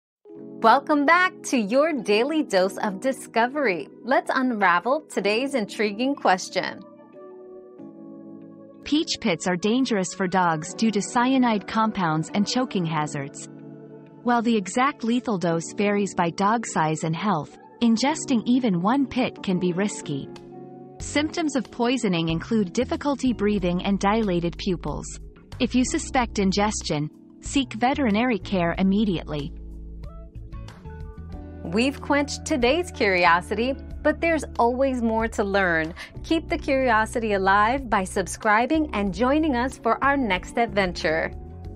How many peach pits are lethal for dogs?
Peach Pits and Dogs: How Many are Lethal? 👉 Peach Pits and Dogs 👉 Discover the hidden danger of peach pits for dogs and learn about the potential lethal effects. Find out why even one pit can pose a serious risk, and the symptoms of poisoning to watch out for. Don't wait, take action and protect your furry friend's health!

Laura S. Harris (2024, January 28.) How many peach pits are lethal for dogs?
    🌐 AskAbout.video